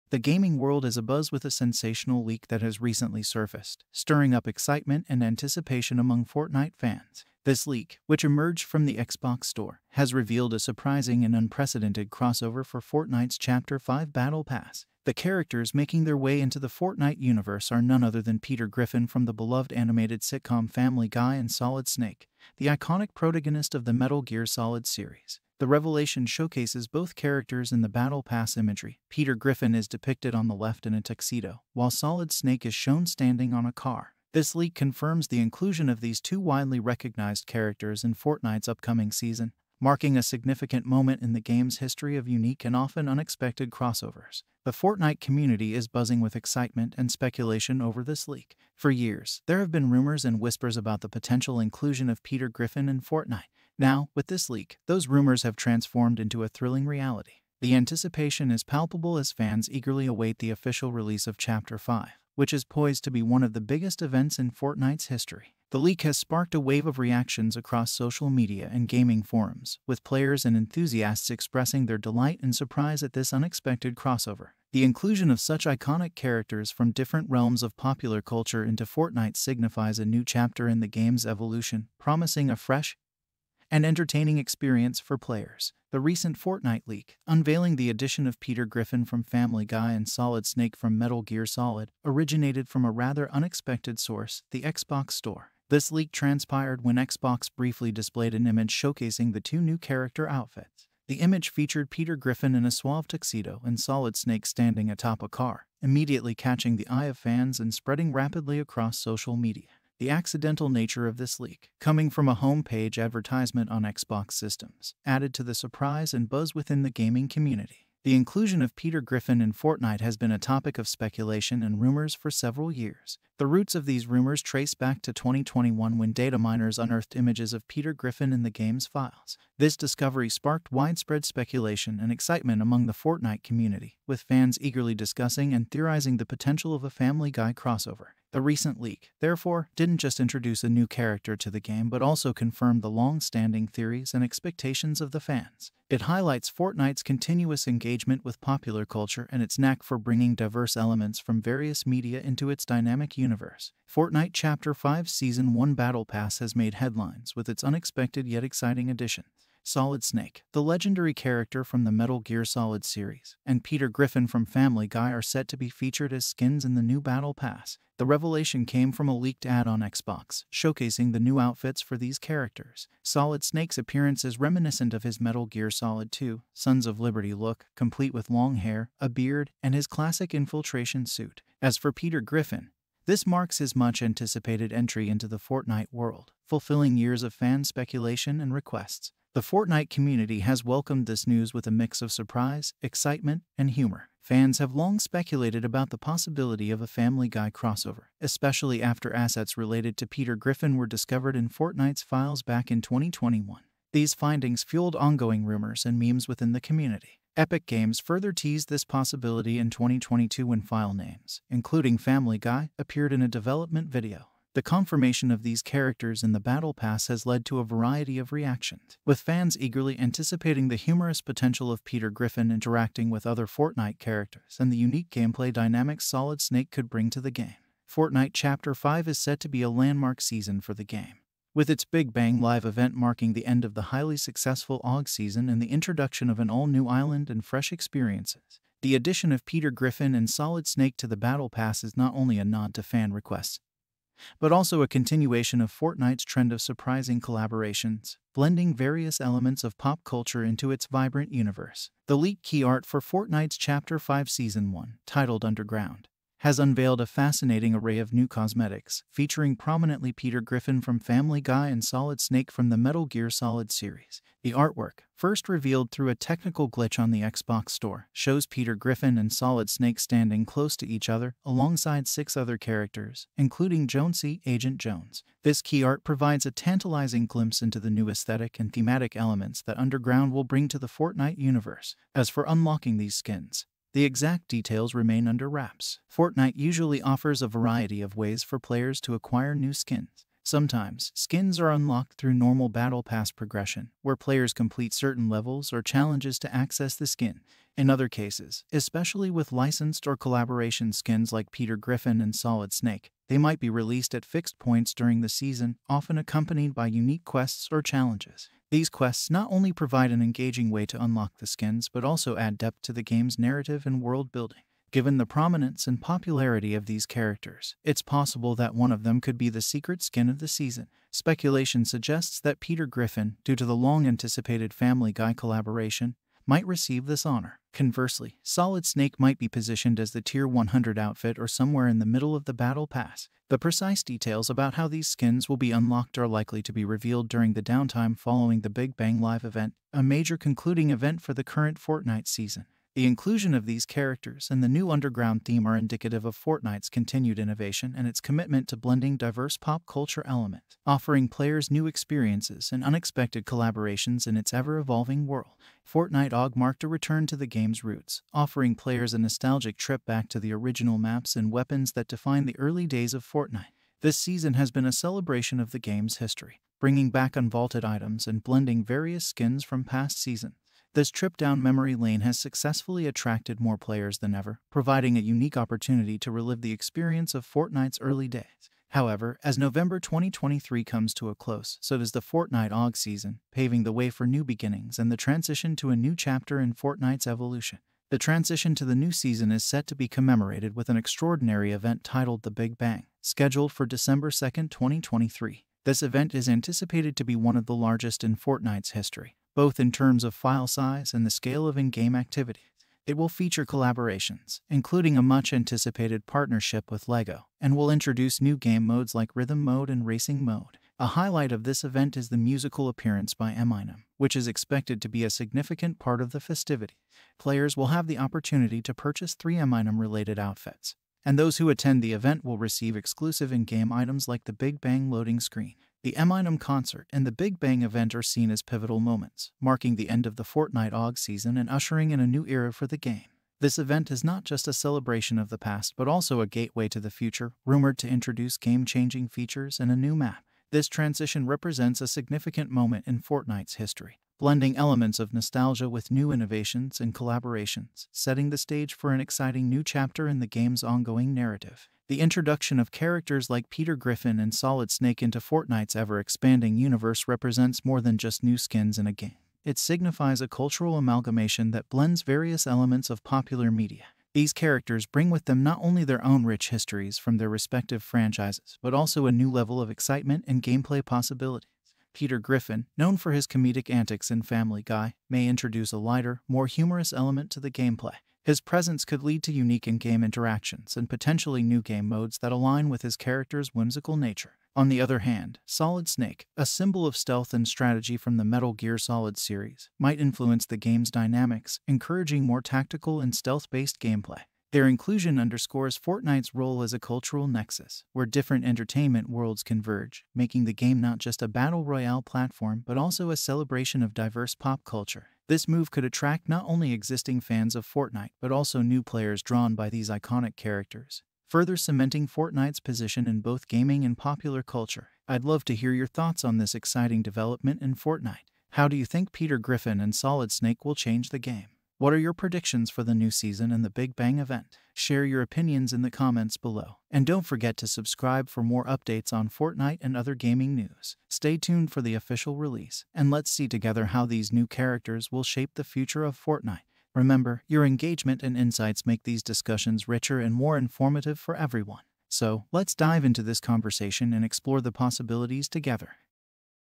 Fortnite Unleashed: Eminem, Peter Griffin & Secrets Revealed!
chapters:
0:00 Introduction
1:25 Leak Origins
2:33 Fortnite Chapter 5 Battle Pass
4:11 Artwork and Skin Details
5:55 Season Transition and Events
7:31 Conclusion